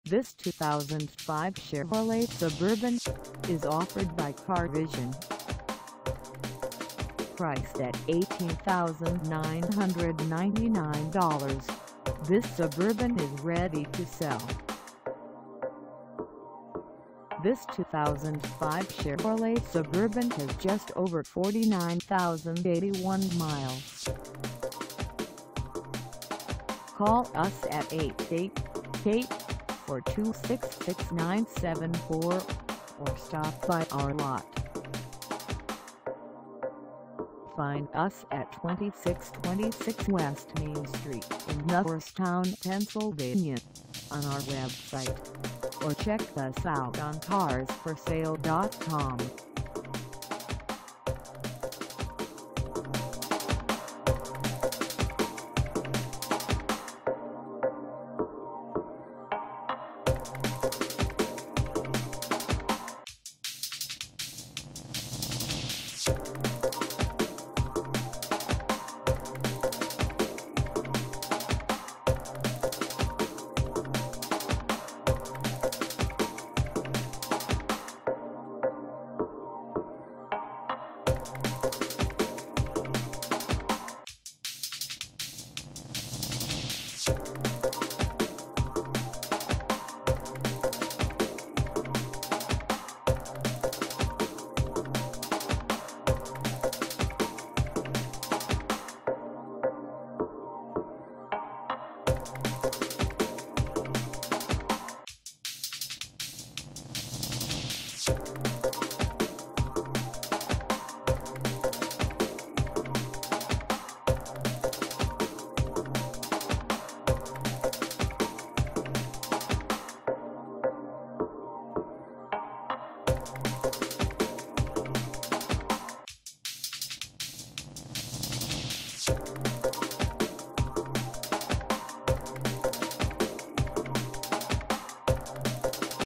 2005 Chevrolet Suburban LT VERY RARE 2500 with 3rd ROW for s
This 2005 Chevrolet Suburban LT VERY RARE 2500 with 3rd ROW is for sale in Norristown, PA 19403 at Car Vision.
Engine: V8 6.0L V8
Transmission: Automatic 4-Speed
Color: BLUE
Mileage: 49081
Contact Car Vision 2626 W Main St Norristown, PA 19403 to test drive this 2005 Chevrolet Suburban LT VERY RARE 2500 with 3rd ROW today.